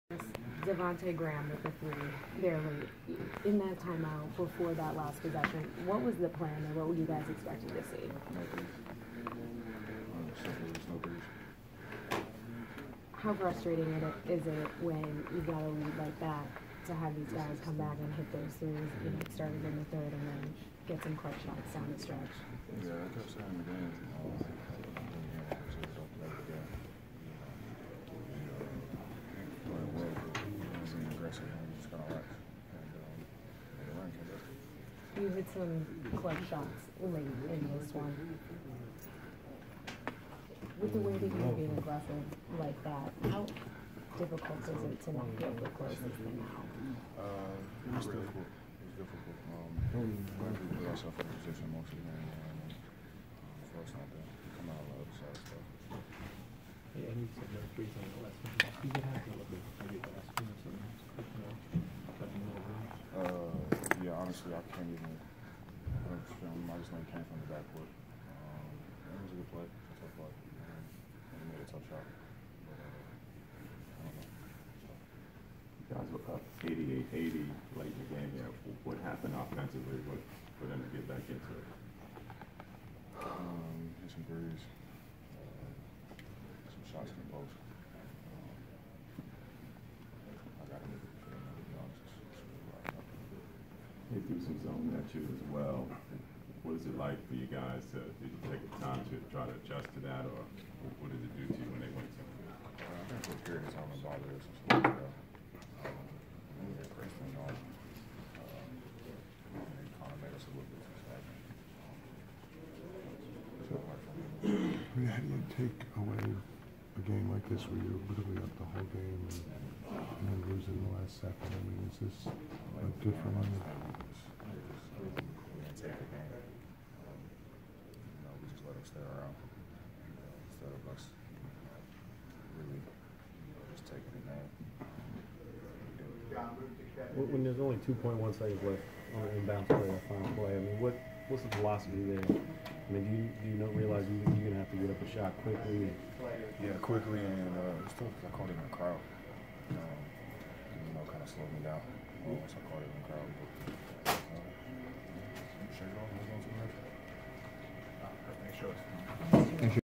Julius Randle discusses attempted game winner that he missed
Julius Randle of the New York Knicks discusses his attempted game winner that he missed, the game-winner by Devonte Graham, and more after the team blows a 15-point lead to lose to the Charlotte Hornets at Madison Square Garden in New York City